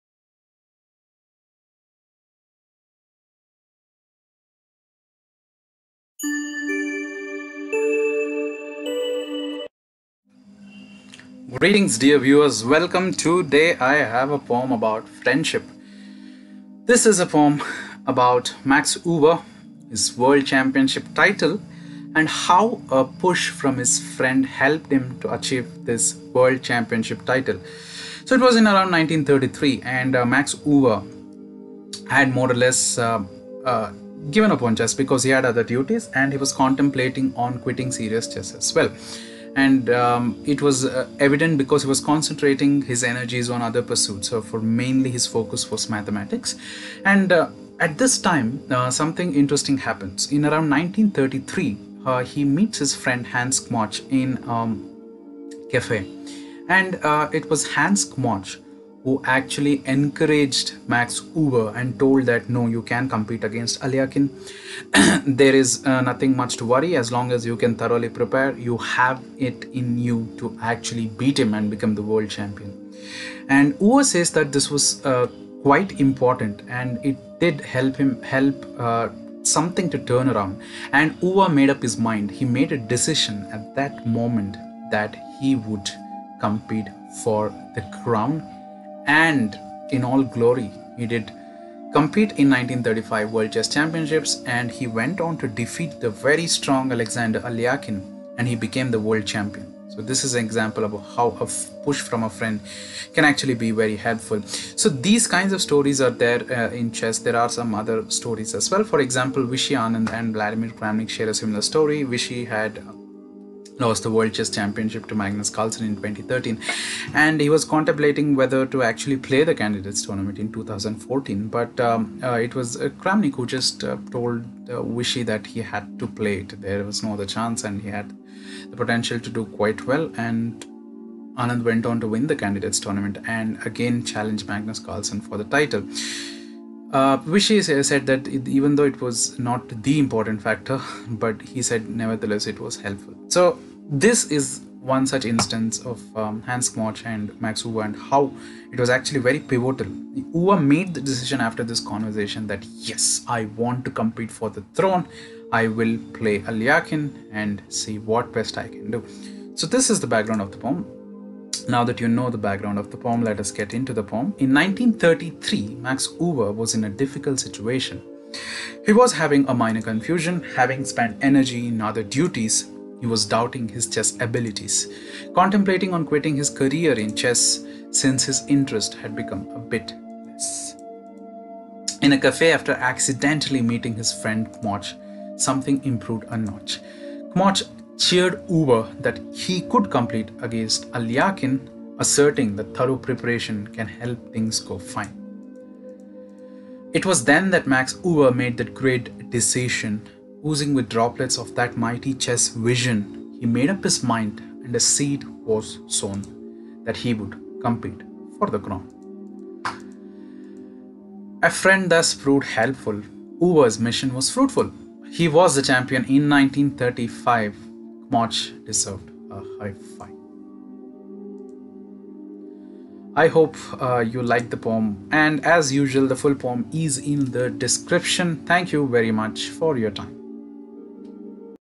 EUWE, KMOCH & FRIENDSHIP!
EUWE,KMOCH AND FRIENDSHIP!

In 1933, Max Euwe was in a difficult situation,
He was having a minor confusion,
Having spend energy in other duties,
He was doubting his chess abilities!
Contemplating on quitting his career in chess,
Since his interest had become a bit less.

In a cafe after accidentally meeting his friend Kmoch(1933),
Something improved a notch,
Kmoch cheered Euwe and stated that he could compete against Alekhine,
Asserting that thorough preparation can help things go fine!

It was then that Max Euwe made that great decision,
Oozing with droplets of that mighty chess vision,
He made up his mind and a seed was sown,
That he would compete for the crown!

A friend thus proved helpful,
Euwe’s mission was fruitful,
He was the champion in 1935!
Kmoch deserved a high- five!

-Chesspoet64